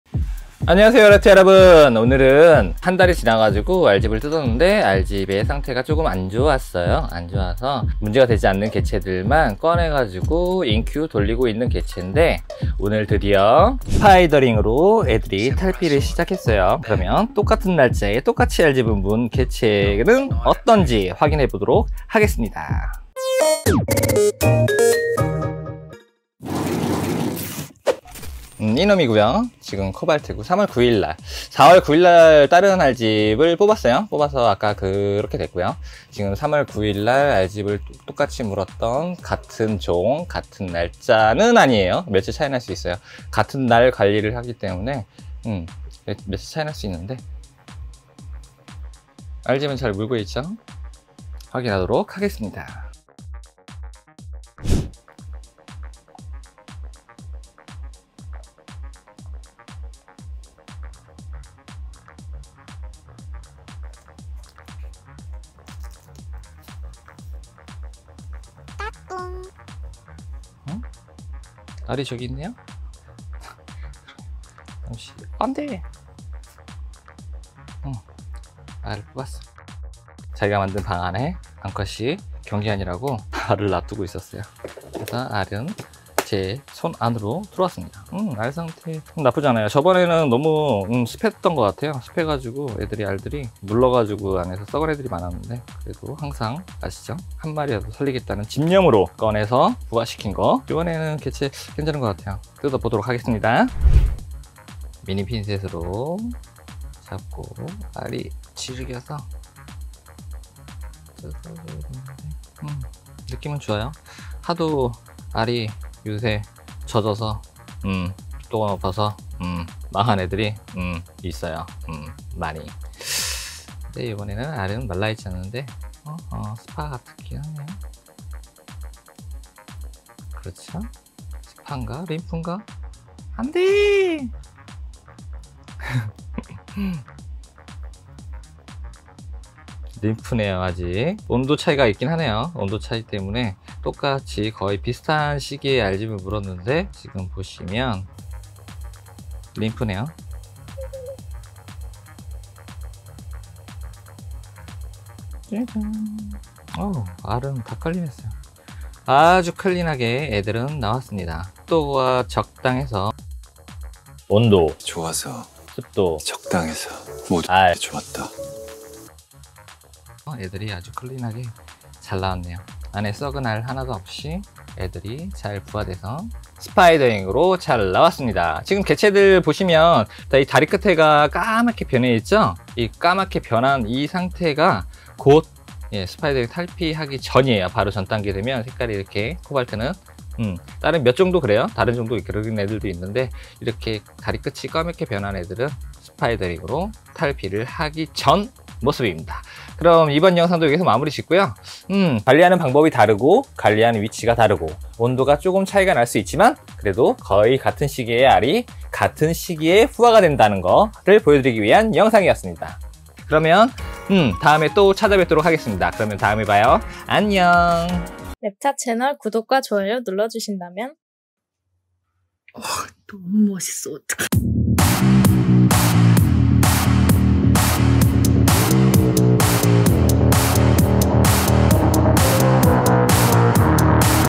코발트 블루 인큐에선 스파이더링 탈피 그럼 어미가 관리하는 애들 상태는?
3월9일 알집(2개) 확인 한 코발트 블루 30일 후 상태 림프 상태, 인큐에서 관리 48일째 스파이더링 탈피, 어미가 관리하고 있는 알집 안의 상태는 ?